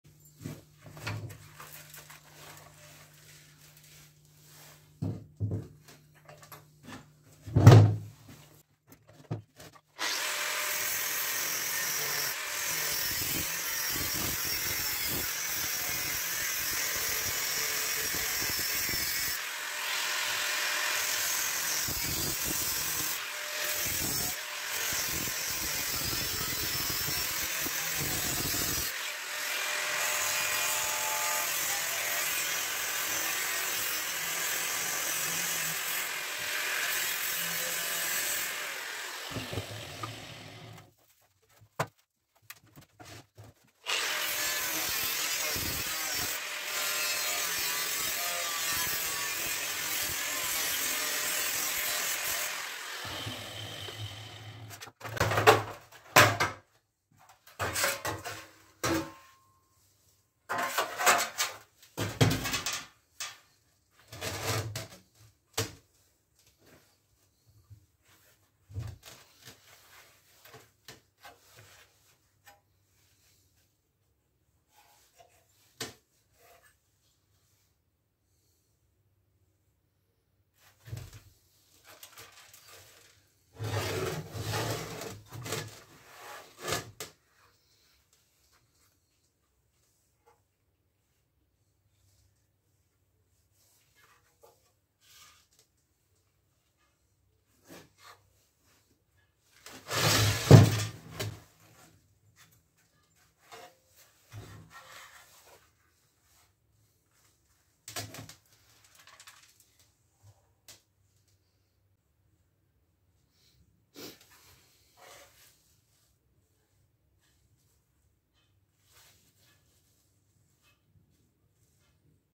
What's Inside a Stock BMW Exhaust Muffler | BMW e39 Muffler
Bmw e39 523i (m52b25) exhaust muffler.